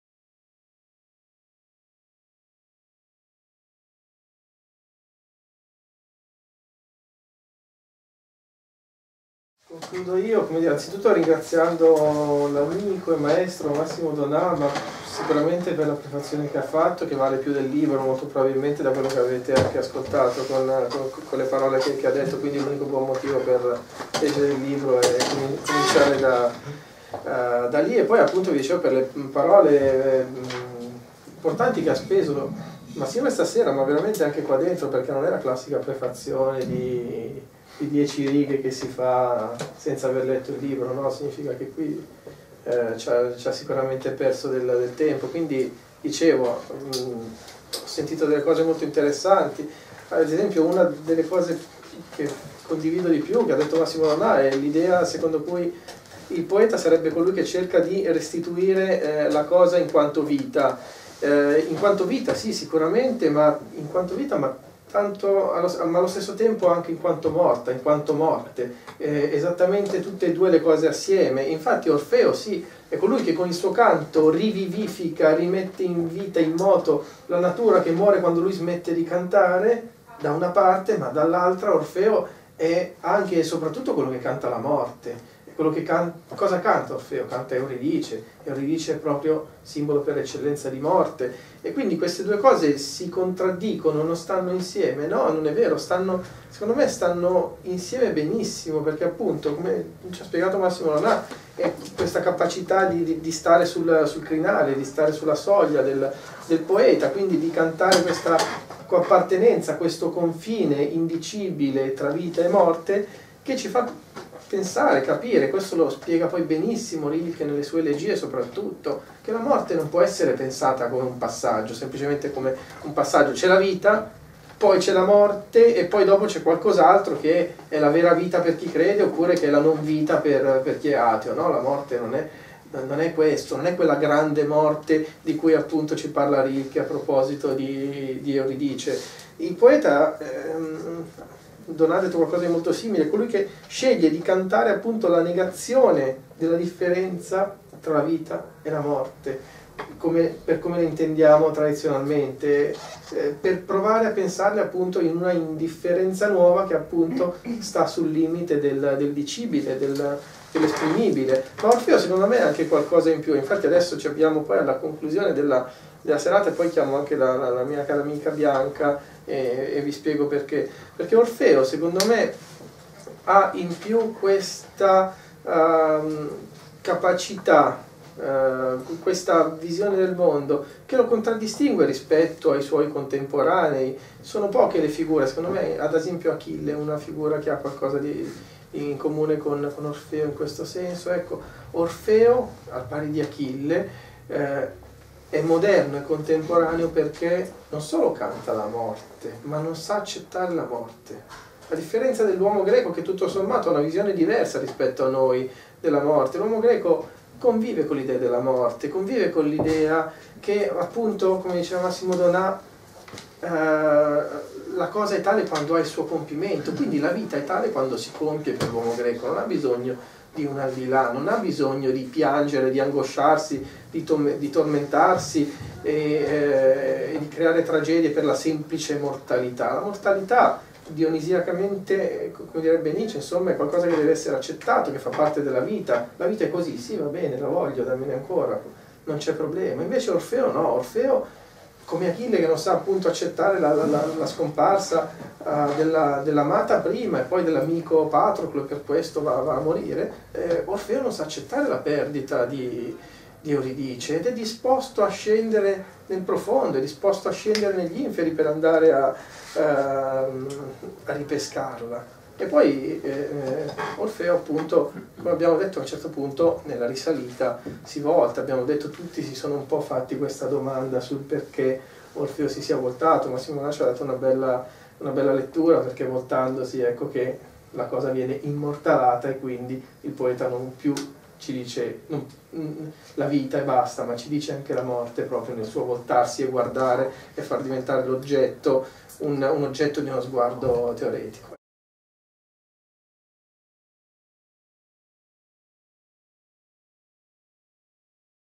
“Meta(m)orphica: poesia e filosofia. Orfeo, Rilke“, con Massimo Donà ed Erasmo Silvio Storace, pt. 3
Presentazione del libro "Meta(m)orphica: poesia e filosofia. Orfeo, Rilke", di Erasmo Silvio Storace (ed. Jouvence), con Massimo Donà ed Erasmo Silvio Storace, presso la Libreria Utopia di Milano. Parte 3: Commento di Erasmo Silvio Storace.